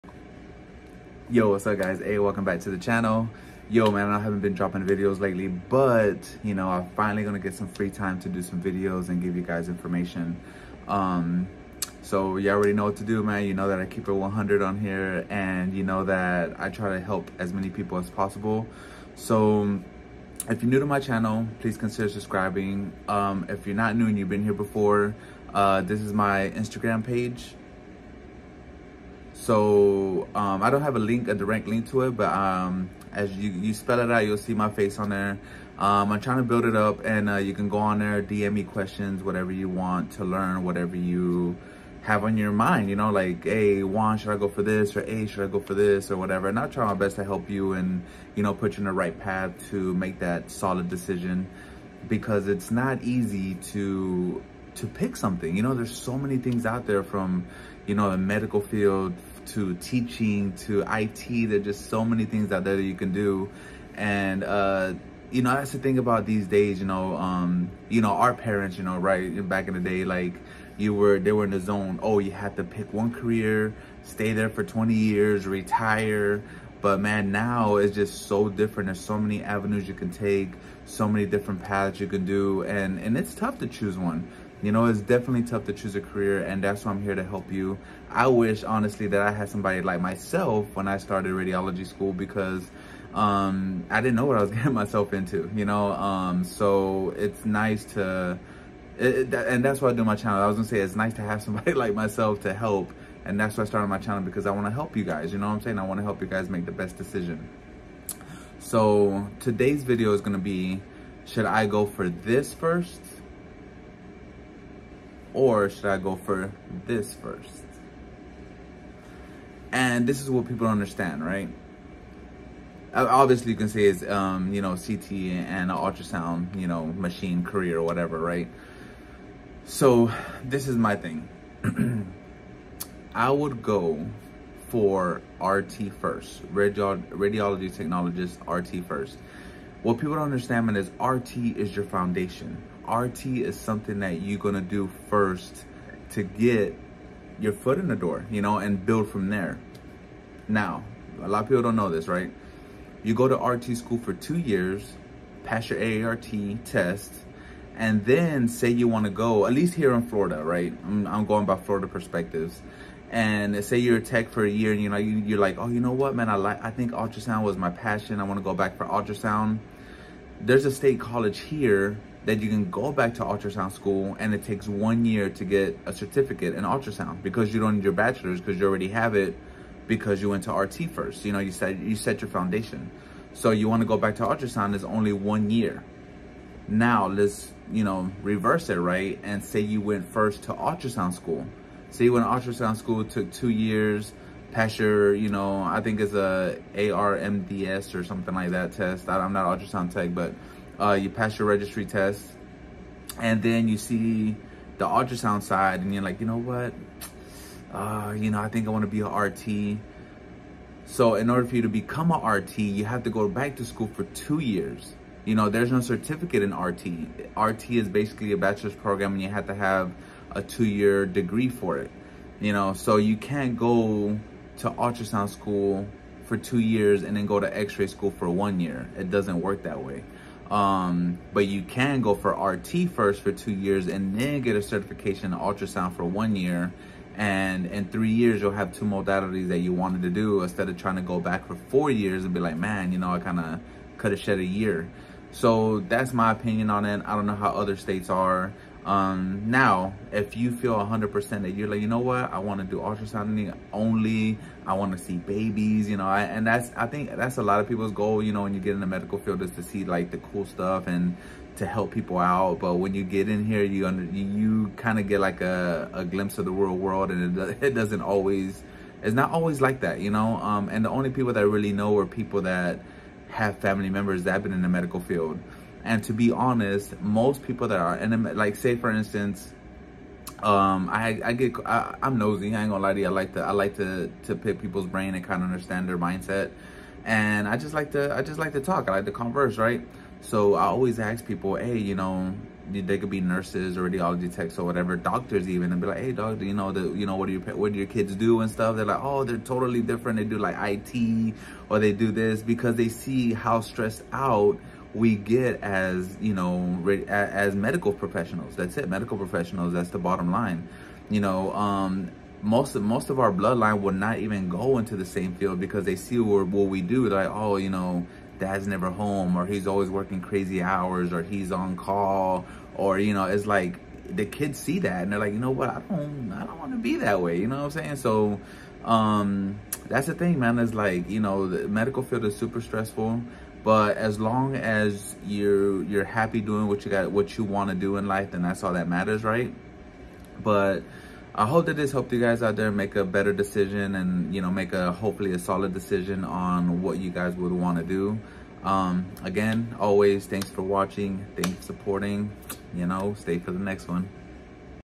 SHOULD I GO FOR ULTRASOUND OR RADIOLOGY TECHNOLOGIST ☢️ 👣
Hey guys on this video I’m helping you make the best possible decision to go for Ultrasound or Radiology Tech first.